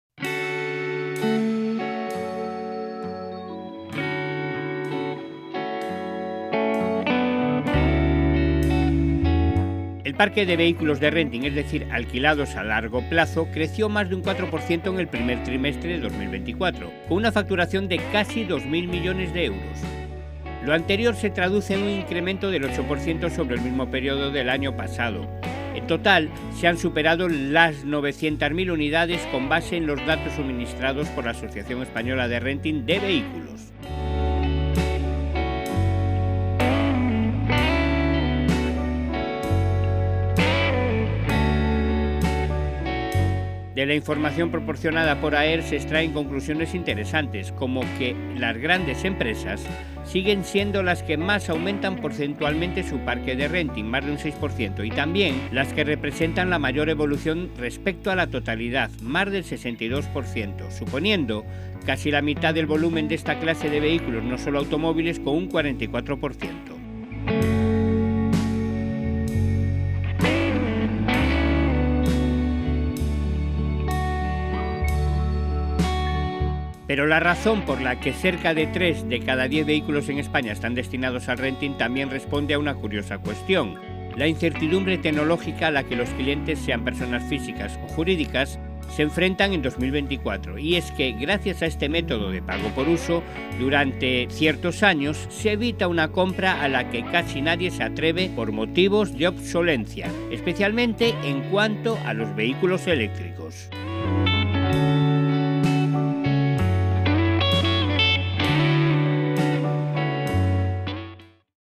¿SE HUYE DE LA COMPRA DE VEHÍCULOS? | By CdRas
El parque de vehículos de renting, es decir, alquilados a largo plazo, creció más de un 4% en el primer trimestre de 2024, con una facturación de casi 2.000 M €. Lo anterior se traduce en un incremento del 8% sobre el mismo periodo del año pasado, 2023. En total, se han superado los 900.000 vehículos con base en los datos suministrados por la Asociación Española de Renting de Vehículos.
De la información proporcionada por AER se extraen conclusiones interesantes, como que las grandes empresas siguen siendo las que más aumentan porcentualmente su parque de renting (+6%) y también las que representan la mayor evolución respecto a la totalidad (+62%), suponiendo casi la mitad del volumen de esta clase de vehículos (no sólo automóviles) con un 44%.
Pero la razón por la que cerca de 3 de cada 10 vehículos en España están destinados al renting también responde a una curiosa cuestión: la incertidumbre tecnológica a la que los clientes, sean personas físicas o jurídicas, se enfrentan en 2024. Y es que, gracias a este método de "pago por uso" durante X años se evita una compra a la que casi nadie se atreve por motivos de obsolescencia, especialmente en cuanto a los vehículos eléctricos.